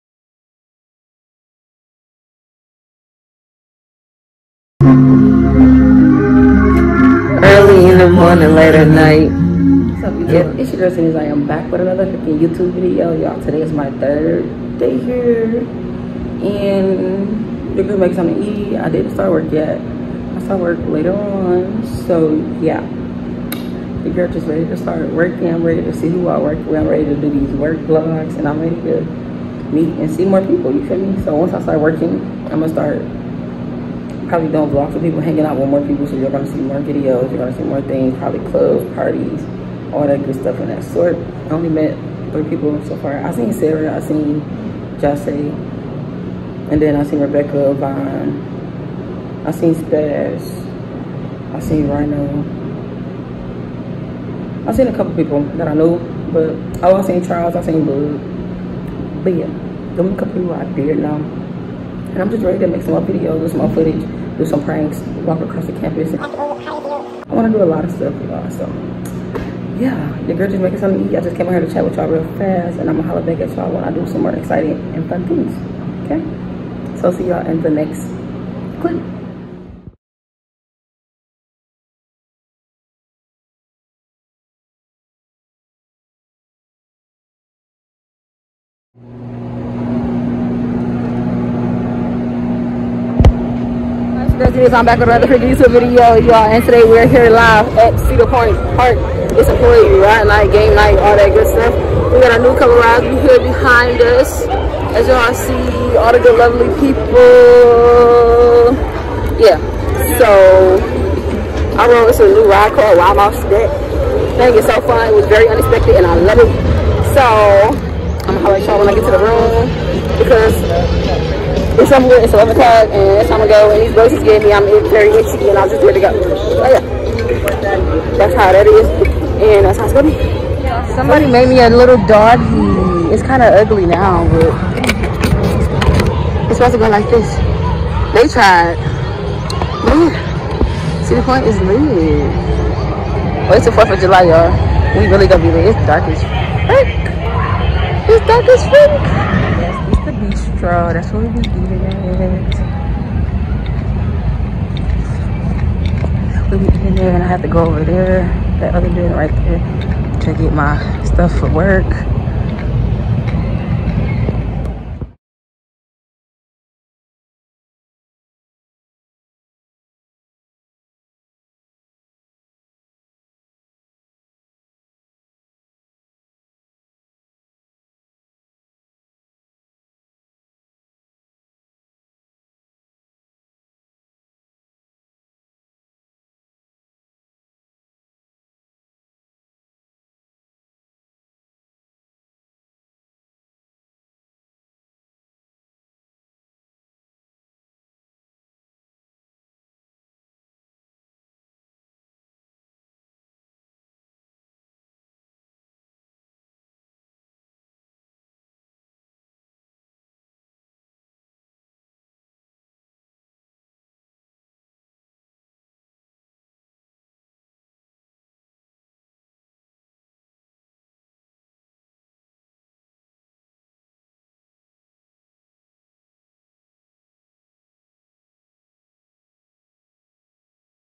Cedar Point Associate Night * Kick-off On The Boardwalk
In today’s video & upcomig videos I will be showing you Cedar Point, Employee Campus, and More as we get into another year here.